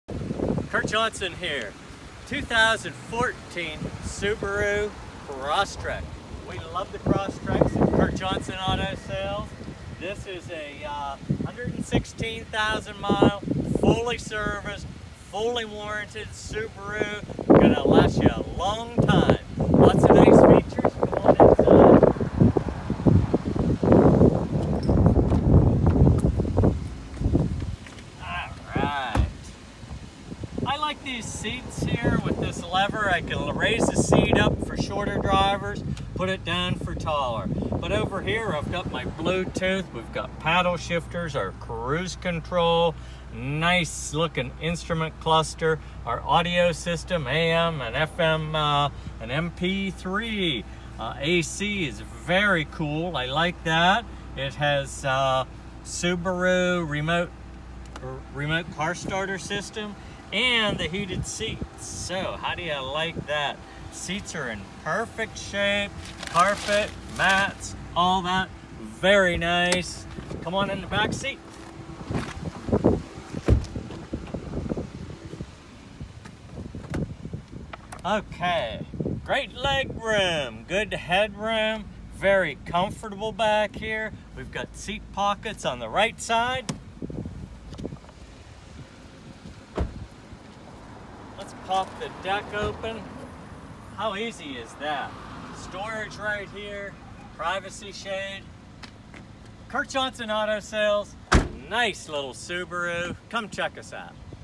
WOW! 2014 Subaru XV Crosstrek 2.0i Premium NOW at Kurt Johnson Auto Sales of DuBois, PA! 9385
The perfect subcompact SUV! This 2014 Subaru XV Crosstrek 2.0i Premium has style, class, comfort, maneuverability and the long-term reliability that Subaru has become known for. Come on down to Kurt Johnson Auto Sales of DuBois, PA today to check it out or give us a ring at !